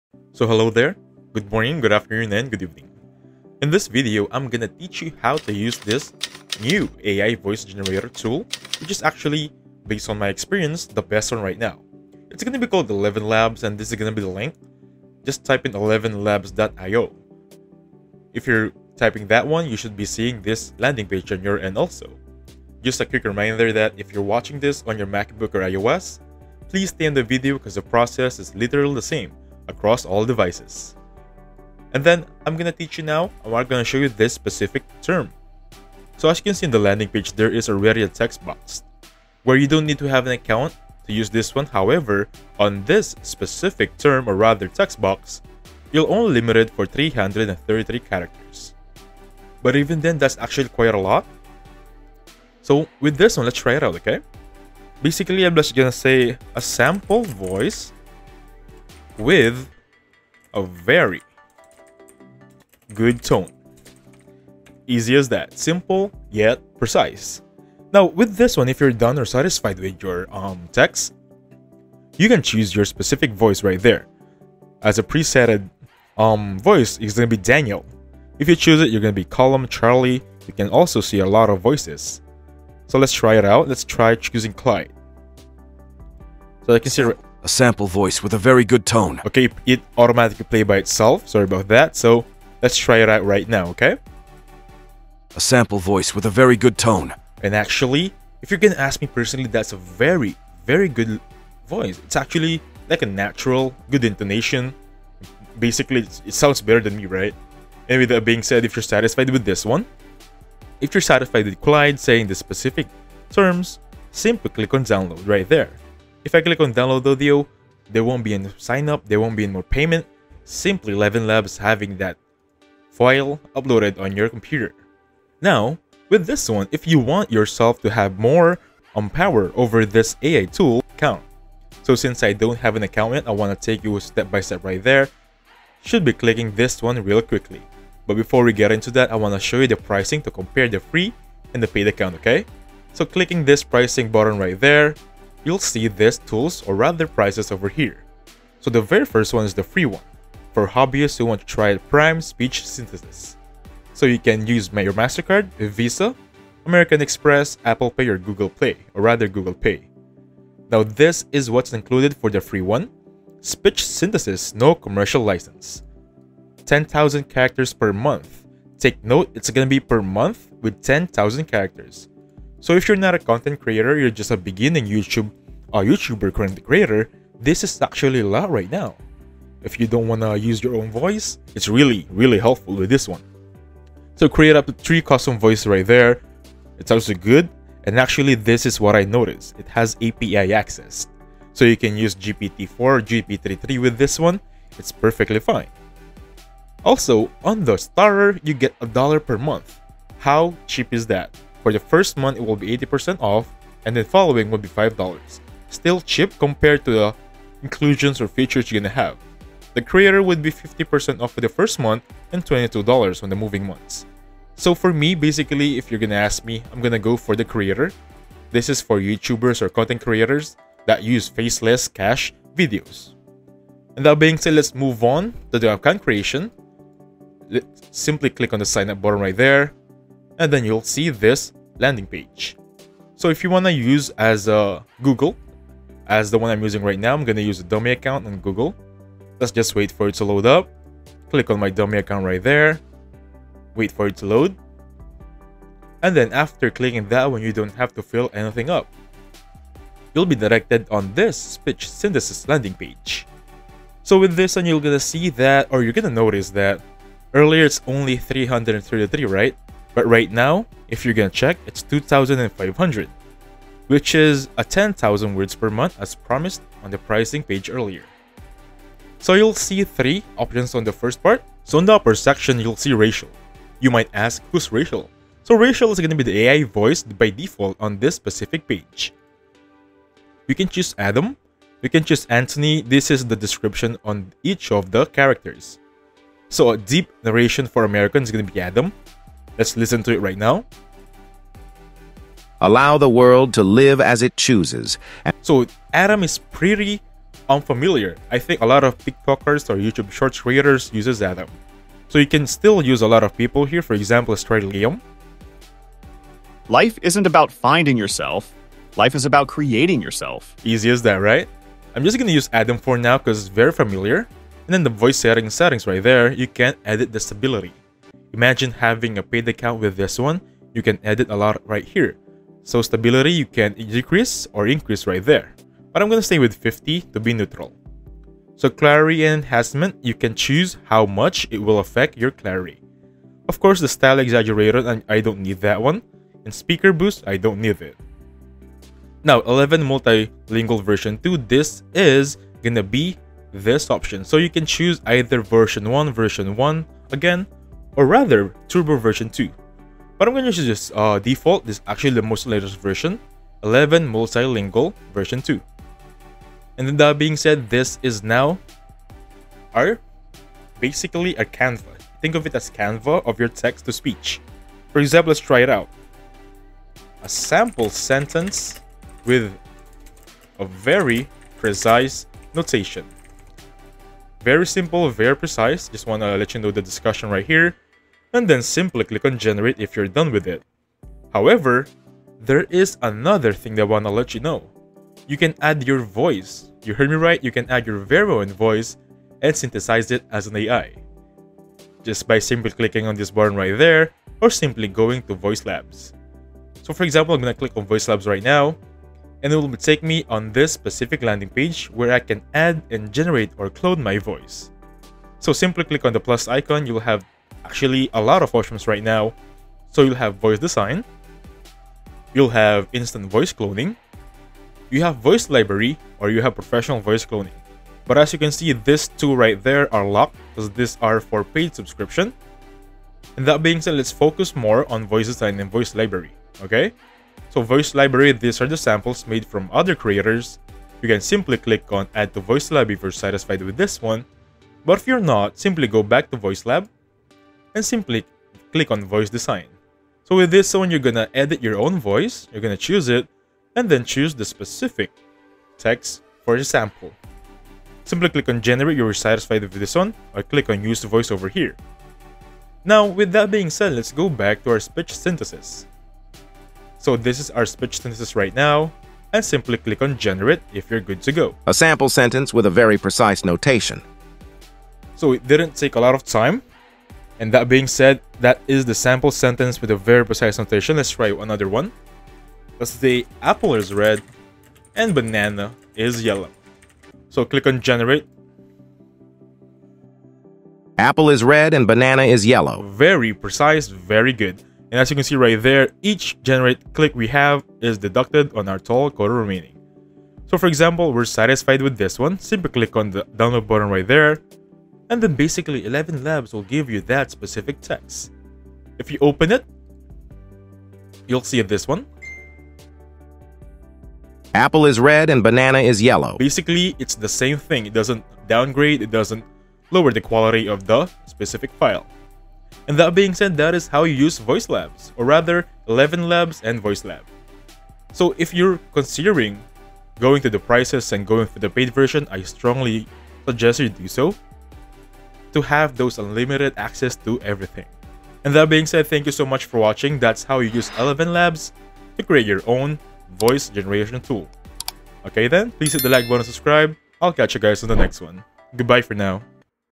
How To Use The BEST AI Voice Generator 2024
WELCOME to our in-depth tutorial on using the BEST AI Voice Generator from Eleven Labs! If you've been searching for a reliable and powerful tool for voice cloning and generation, you're in the right place.

Join us as we guide you through the process of harnessing the capabilities of Eleven Labs' AI Voice Generator. From cloning voices to crafting original content, this tutorial covers everything you need to know.

Elevate your content creation with the BEST AI Voice Generator from Eleven Labs!